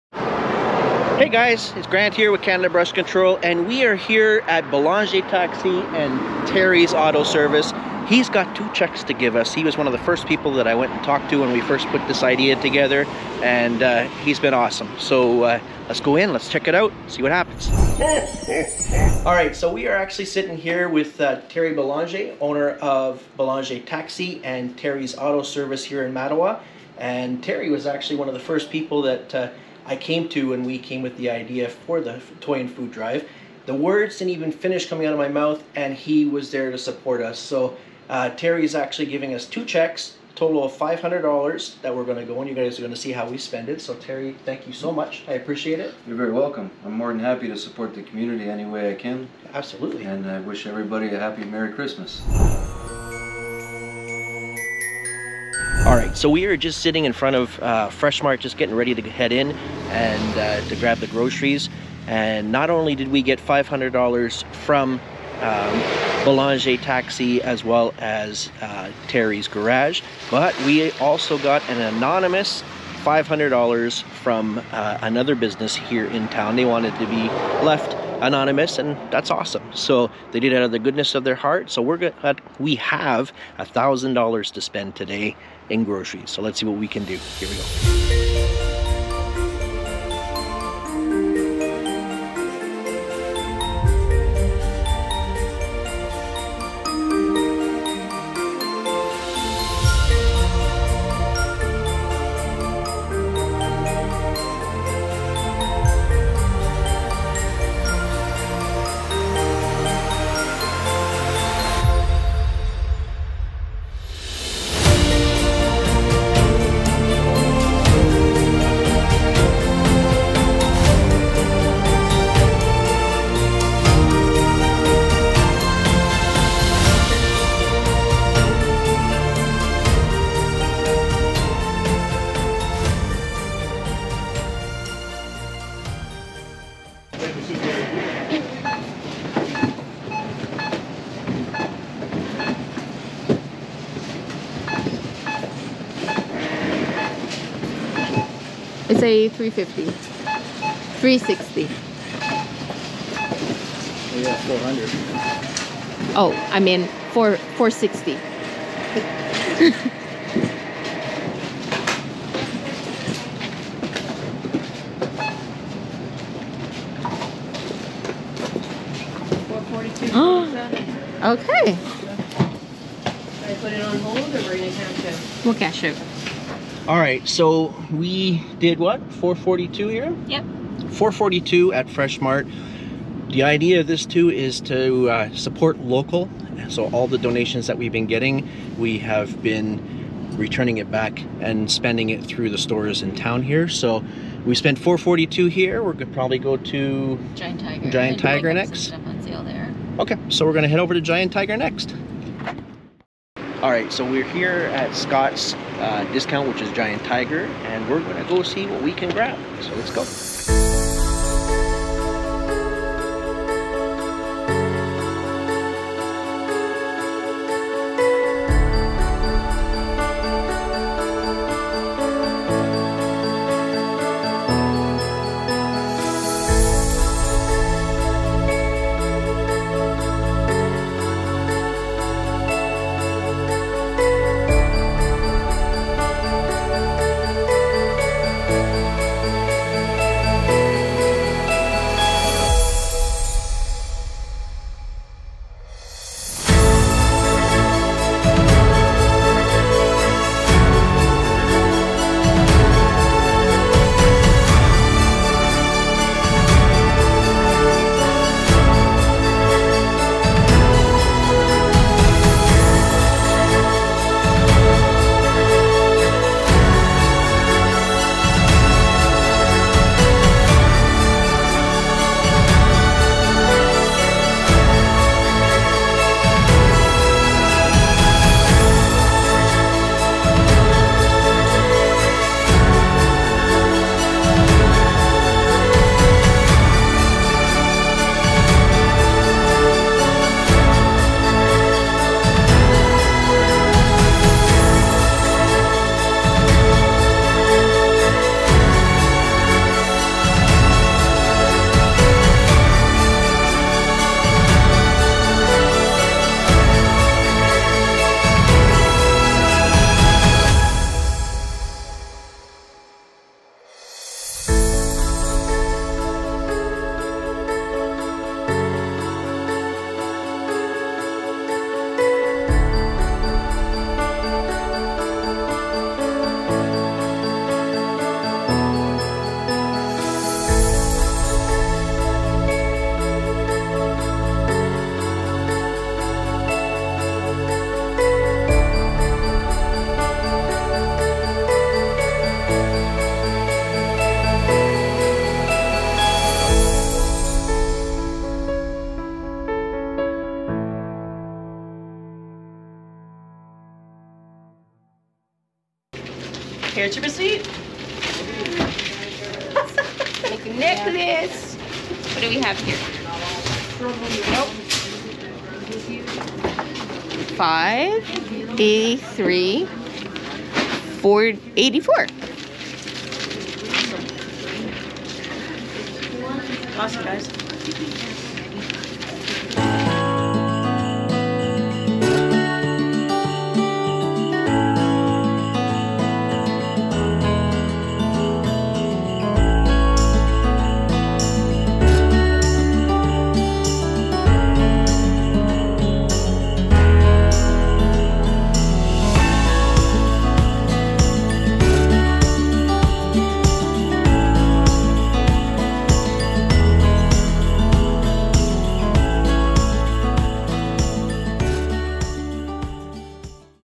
Food Drive shopping spree - Thank you Belanger Taxi!
Thank you Terry Belanger (Belanger Taxi & Terry's Auto Service) for the support and your generous donation for the upcoming Toy & Food Drive happening on Saturday, December 9th, from 10am - 4:30pm in front of Wilsons Builders Supplies Home Hardware!

We'd also like to thank a business owner in town as well who chose to stay anonymous, but donated a generous donation as well.

Together as a community, we can make a difference!